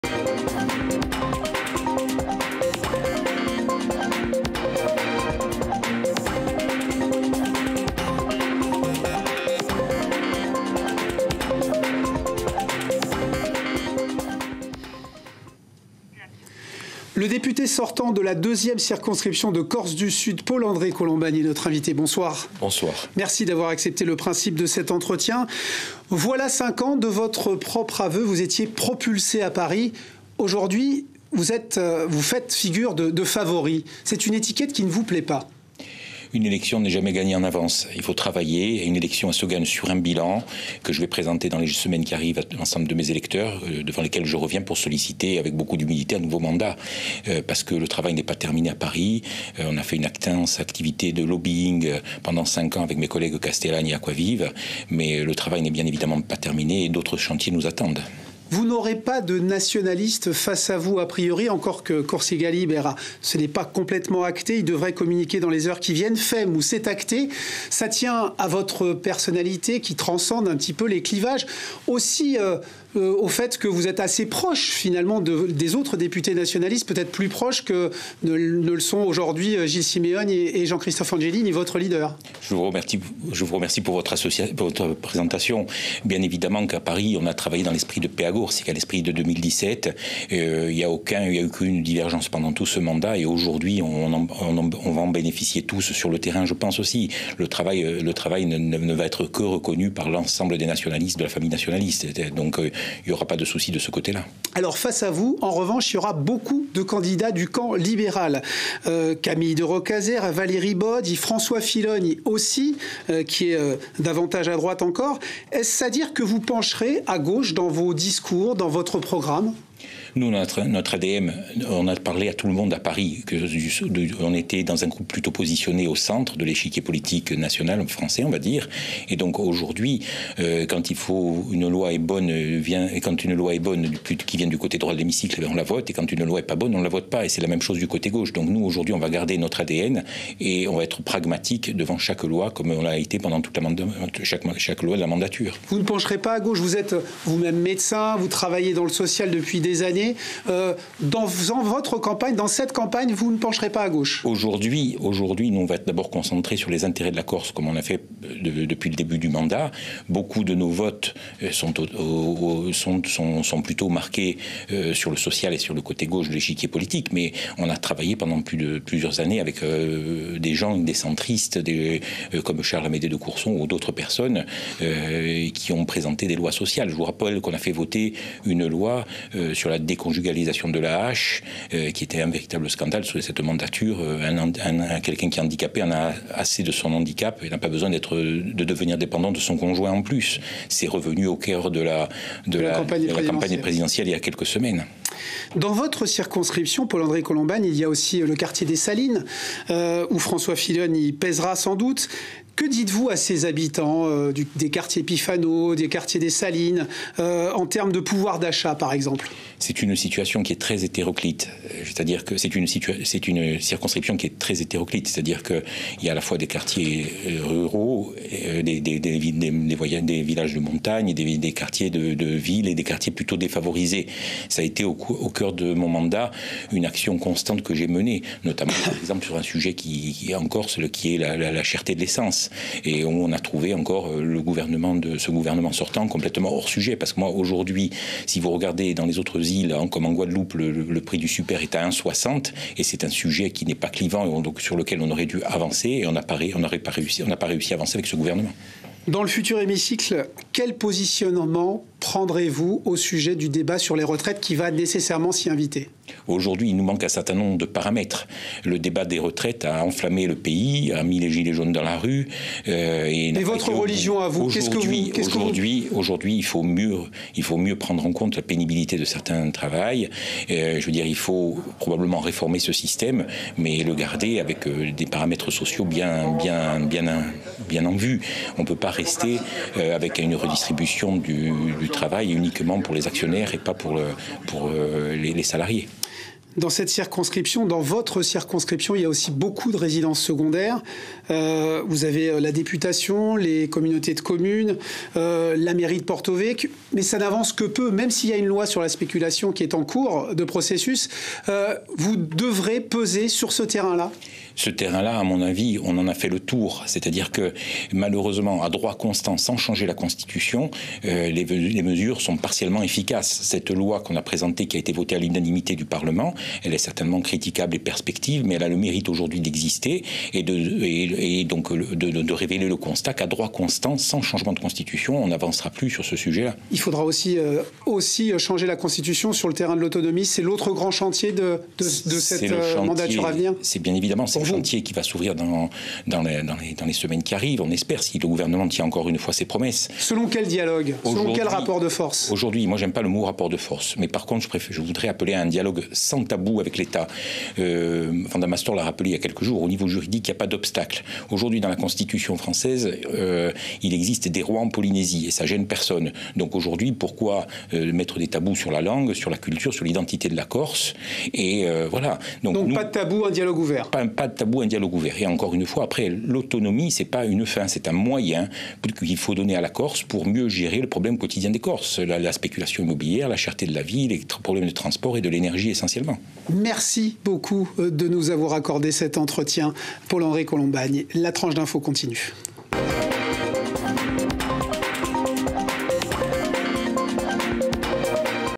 In Tantu : Elezzione Legislative 2022 : Paul-André Colombani
Entretient avec Laurent Vincensini